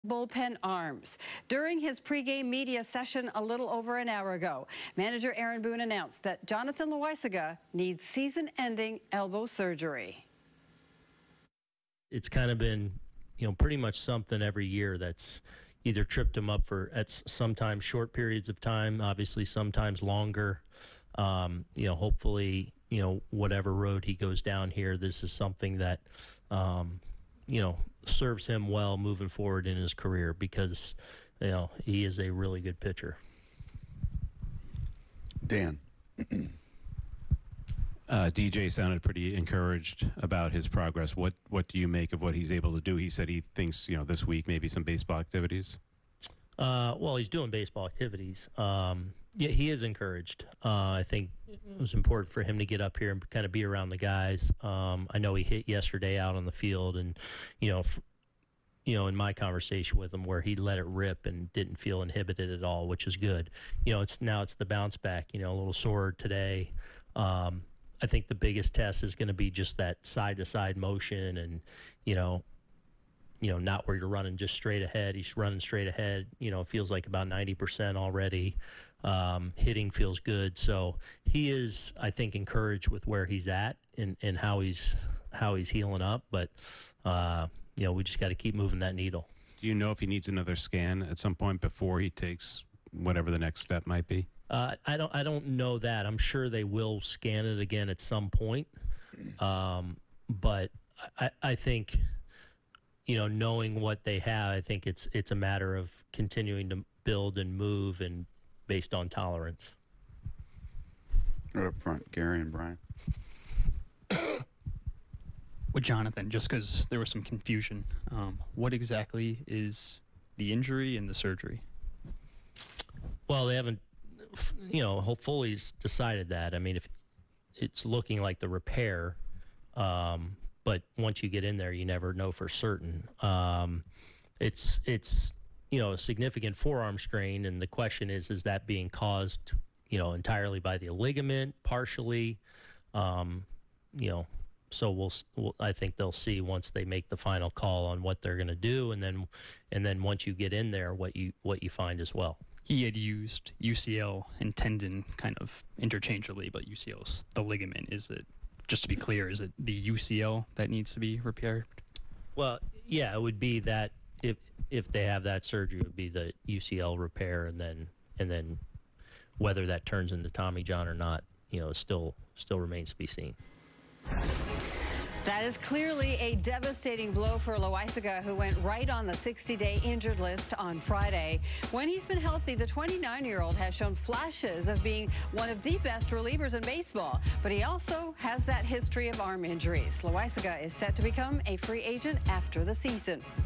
Jonathan Loáisiga needs season-ending surgery, per Aaron Boone
Jonathan Loáisiga needs season-ending surgery. Aaron Boone with more on Loáisiga and DJ LeMahieu.